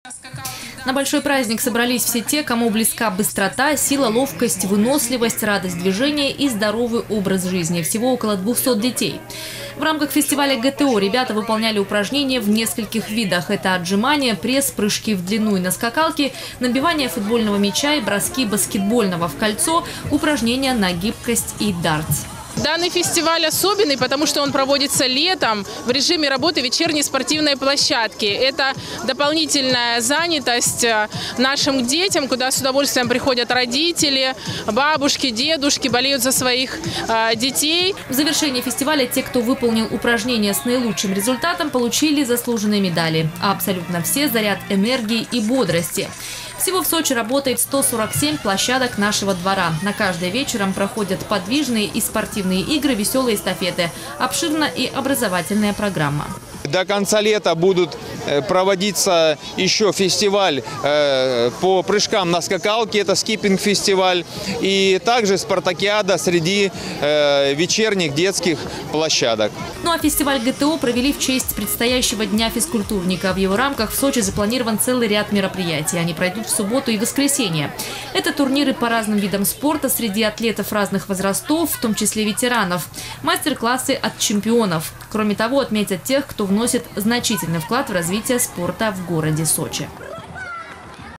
В Адлере прошел Фестиваль ГТО. Новости Эфкате Сочи
Провели его в Адлере в честь предстоящего Дня физкультурника, который по всей стране будут праздновать в эту субботу.

На большой праздник собрались все те, кому близка быстрота, сила, ловкость, выносливость, радость движения и здоровый образ жизни. Всего около 200 детей. В рамках фестиваля ГТО ребята выполняли упражнения в нескольких видах. Это отжимание, пресс, прыжки в длину и на скакалке, набивание футбольного мяча и броски баскетбольного в кольцо, упражнение на гибкость и дартс.